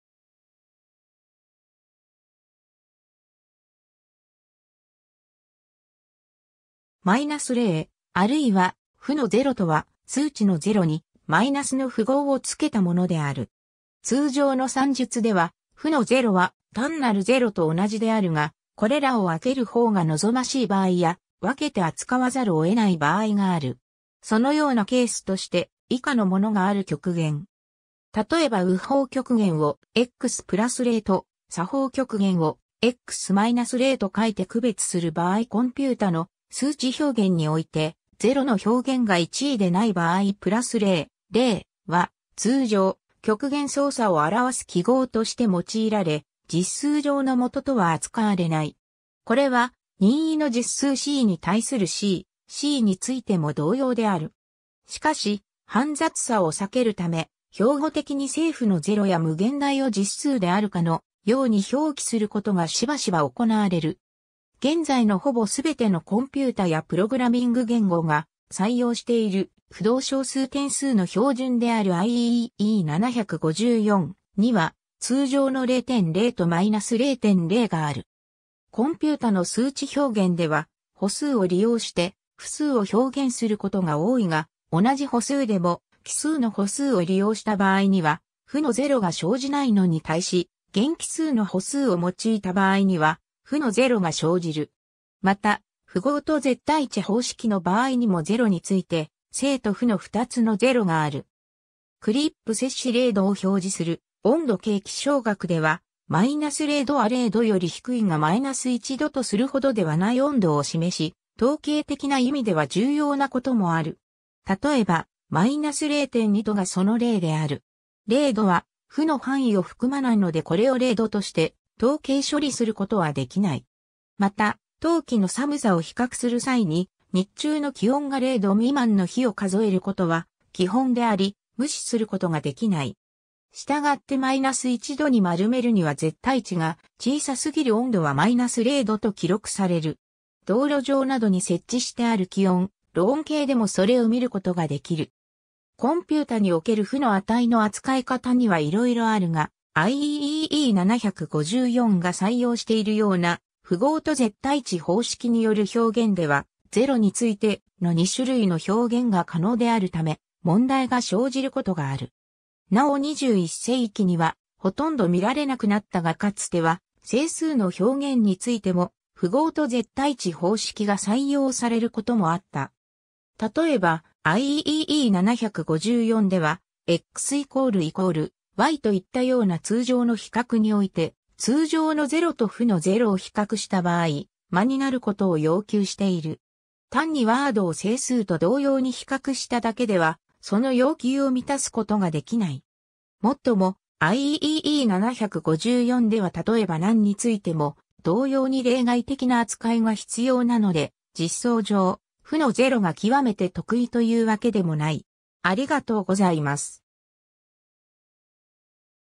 −0
−0（マイナスゼロ）、あるいは負のゼロとは、数値のゼロにマイナスの符号をつけたものである。
 通常の算術では、負のゼロは単なるゼロ（及び正のゼロ、+0）と同じであるが、これらを分ける方が望ましい場合や、分けて扱わざるを得ない場合がある。
 そのようなケースとして、以下のものがある 極限。
例えば右方極限を x → +0 と左方極限を x → −0 と書いて区別する場合 コンピュータの数値表現においてゼロの表現が一意でない場合 +0, −0, +∞, −∞ は通常、極限操作を表す記号として用いられ、実数上の元とは扱われない。
これは任意の実数 c に対する +c, −c についても同様である。
 しかし、煩雑さを避けるため、標語的に正負のゼロや無限大を実数であるかのように表記することがしばしば行われる。
 現在のほぼ全てのコンピュータやプログラミング言語が採用している浮動小数点数の標準である IEEE 754 には通常の 0.0 と -0.0 がある。
 コンピュータの数値表現では、補数を利用して負数を表現することが多いが、同じ補数でも基数の補数を利用した場合には負のゼロが生じないのに対し、減基数の補数を用いた場合には負のゼロが生じる。
また「符号と絶対値」方式の場合にもゼロについて正と負の2つのゼロがある（前述のIEEE 754など）。
 Clip -0 ℃を表示する温度計 気象学では、-0度は 0度（セルシウス温度）より低いが -1度とするほどではない温度を示し、統計的な意味（つまり1度単位で統計を取る場合）では重要なこともある。
例えば、-0.2度がその例である。
0度は負の範囲を含まないのでこれを 0度として統計処理することはできない。
また、冬季の寒さを比較する際に日中の気温が 0度未満（氷点下）の日を数えることは基本であり、無視することができない。
従って -1度に丸めるには絶対値が小さすぎる温度は -0度 と記録される。
道路上などに設置してある気温・路温計でもそれを見ることが出来る。
 コンピュータにおける負の値の扱い方にはいろいろあるが、IEEE 754 が採用しているような「符号と絶対値」方式による表現では、ゼロについて、 の二種類の表現が可能であるため、問題が生じることがある。
なお21世紀にはほとんど見られなくなったがかつては、整数の表現についても「符号と絶対値」方式が採用されることもあった。
 例えば、IEEE 754 では、x==y といったような通常の比較において、通常のゼロと負のゼロを比較した場合、真になることを要求している。
単にワードを整数と同様に比較しただけでは、その要求を満たすことができない。
もっとも、IEEE 754 ではたとえば NaN についても同様に例外的な扱いが必要なので ( NaN==NaN は常に偽で NaN!=NaN は常に真) 、実装上、負のゼロがきわめて特異というわけでもない（IEEE 754 では）。